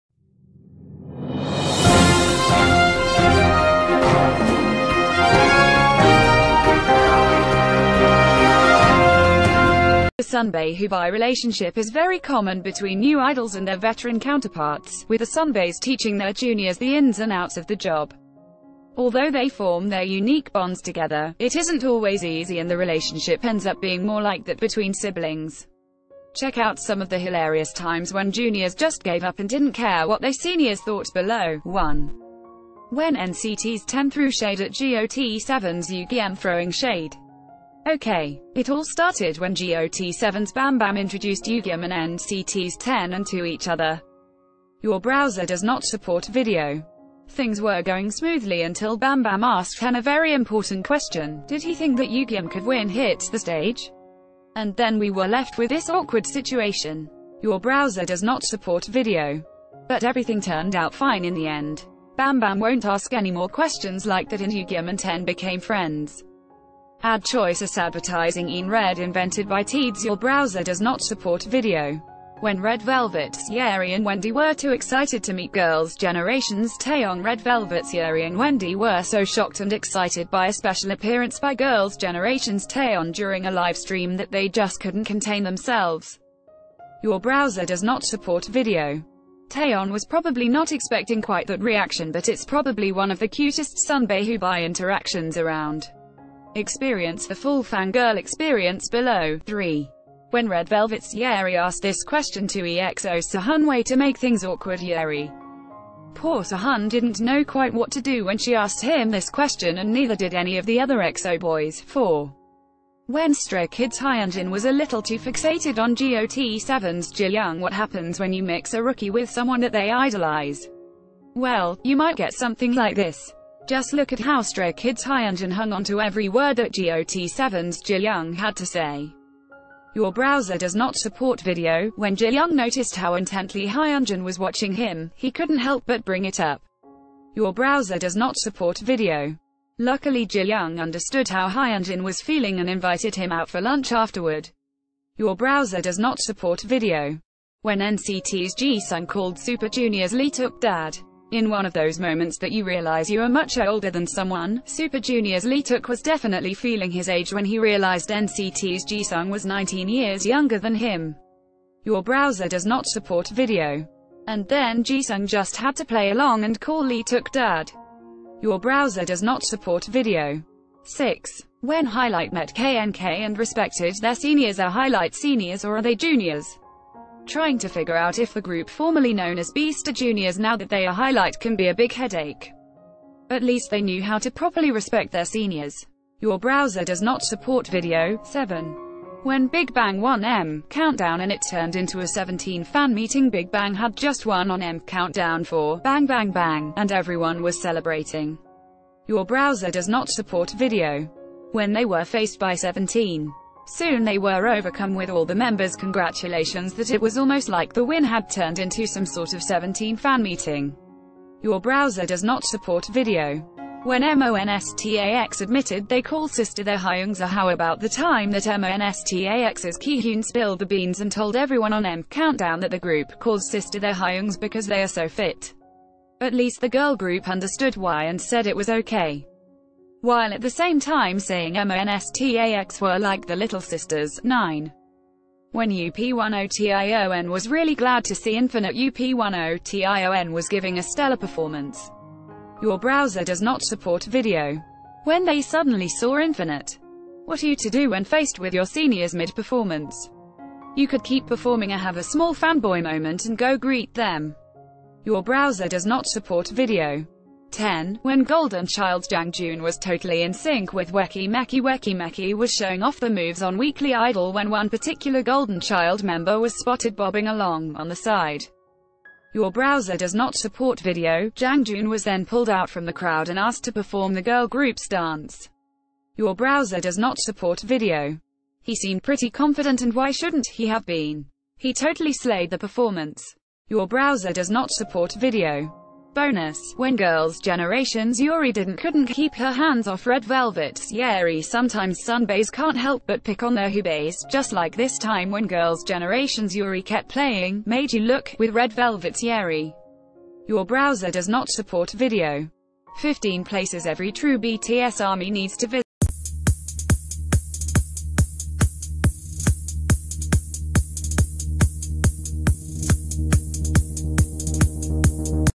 10 Rookies Who Don’t Give AF About Their Seniors
10 Rookies Who Don’t Give AF About Their Seniors
The sunbae-hoobae relationship is very common between new idols and their veteran counterparts, with...